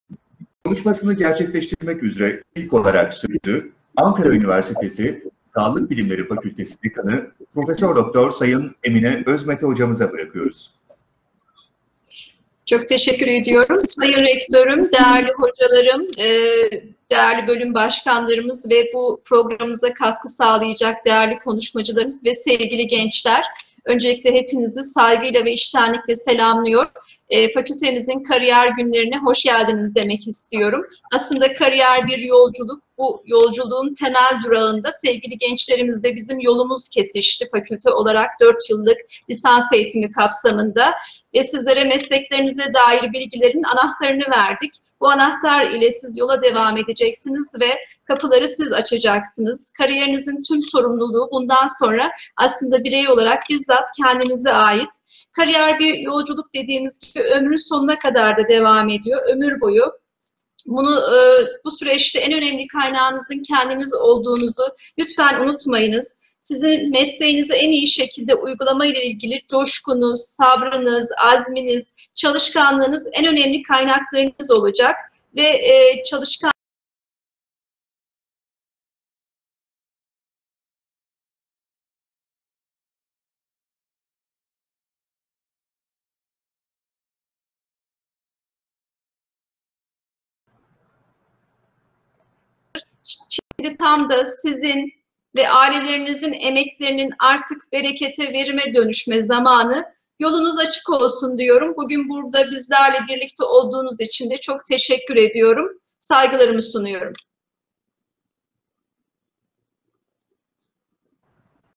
KARİYER GÜNLERİ "Hayallerin Ötesinde": Açılış / Prof. Dr. Emine ÖZMETE
21 Mayıs 2021 tarihinde Ankara Üniversitesi Sağlık Bilimleri Fakültesi tarafından düzenlenen Kariyer Günleri Hayallerin Ötesinde programında Sağlık Bilimleri Fakültesi Dekan V. Prof. Dr. Emine Özmete açış konuşmalarını gerçekleştirdiler.